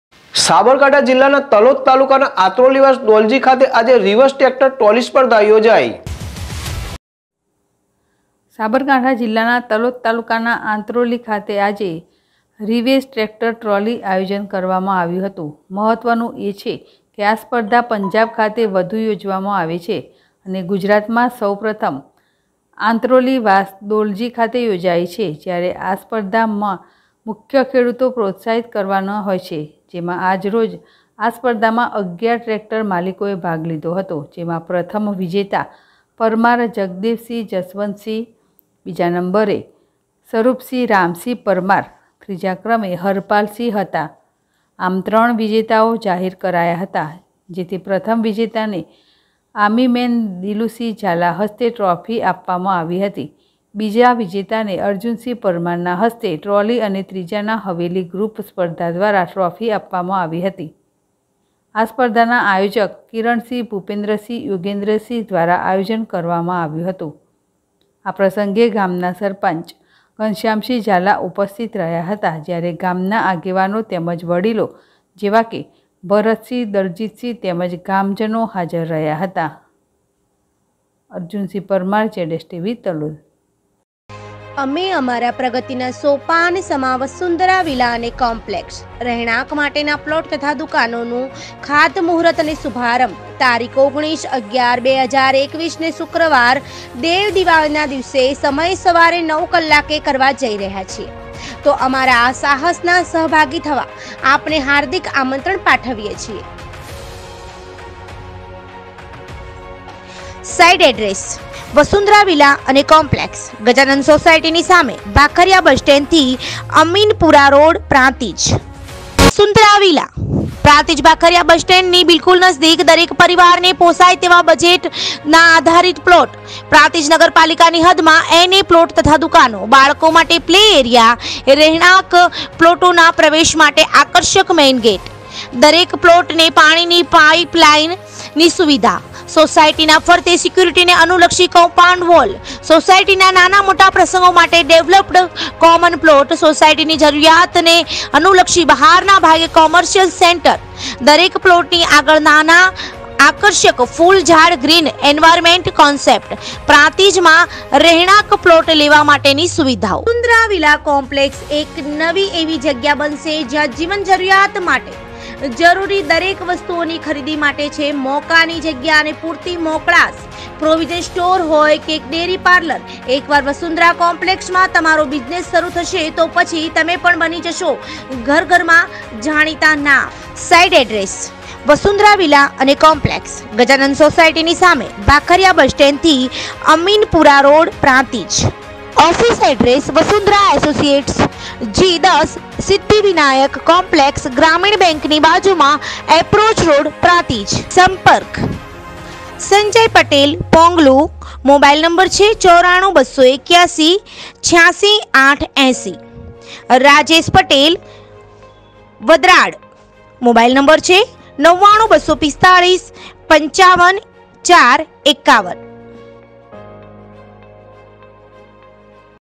તલોદ તાલુકાના આંતરોલી વાસ દોલજી ખાતે આજે રિવૅસ ટ્રેકટર ટ્રોલી સ્પઘૉ યોજાઈ
તલોદ તાલુકાના આંતરોલી વાસ દોલજી ખાતે આજે રિવૅસ ટ્રેકટર ટ્રોલી સ્પઘૉ યોજાઈ//ZSTVNEWS//